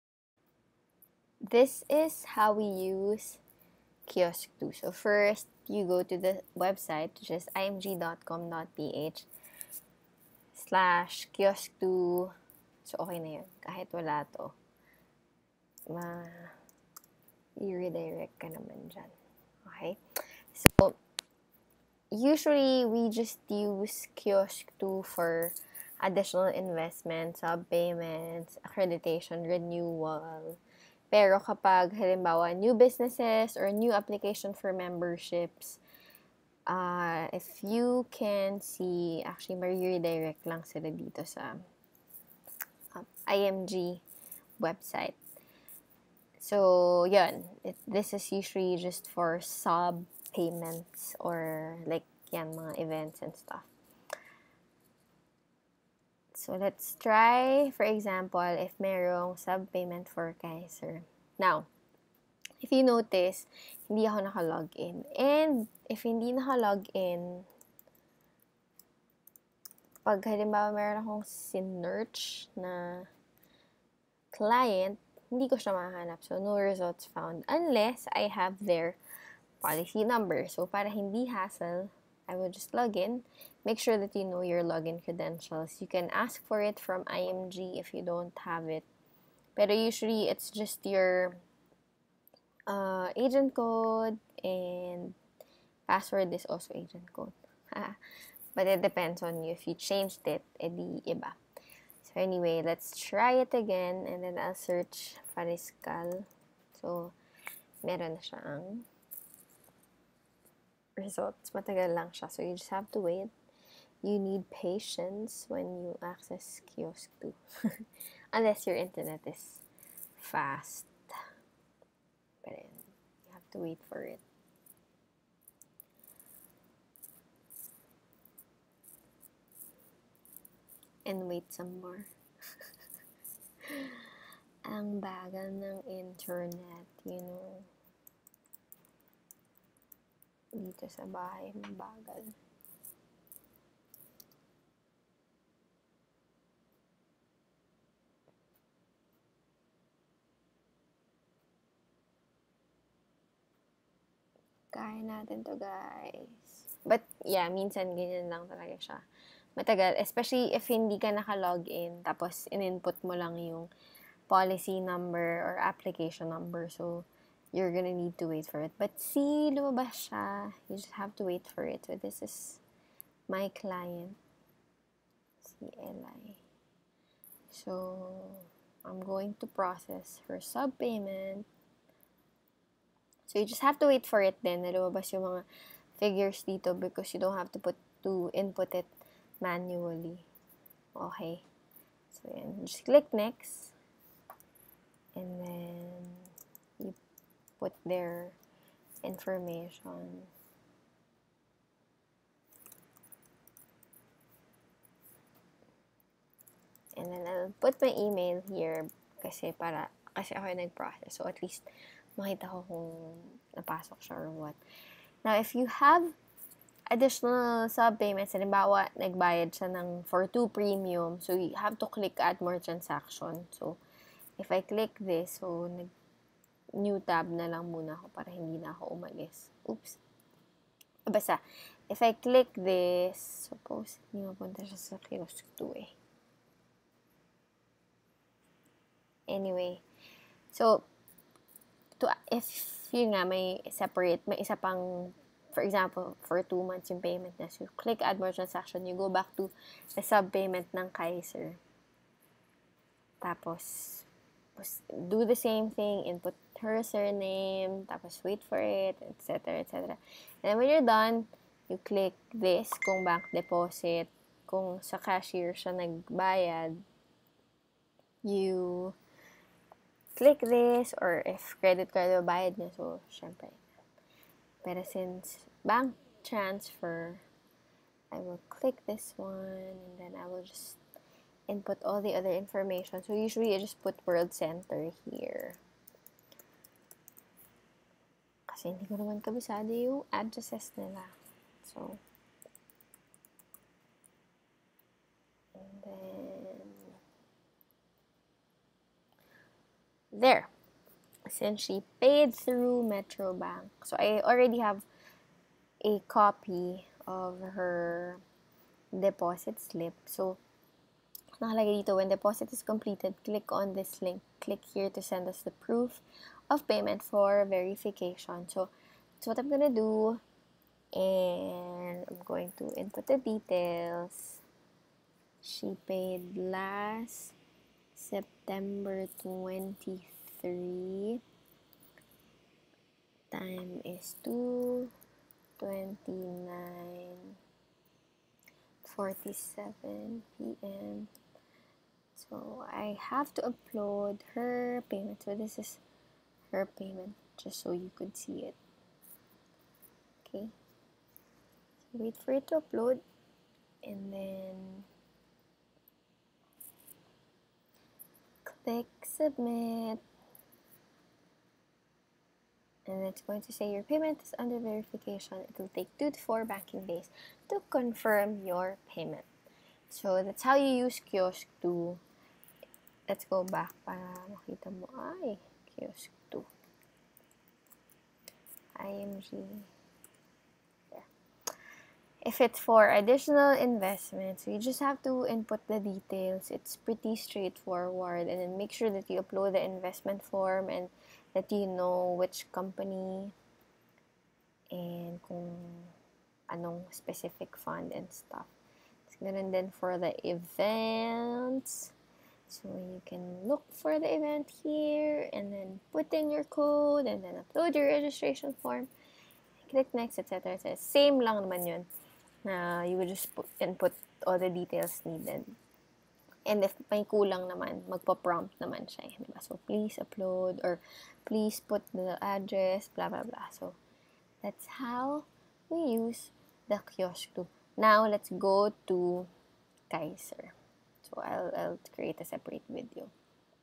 How To Use IMG Kiosk2
Here's a video on how to use Kiosk2 for Kaiser sub-payments, additional investments, etc.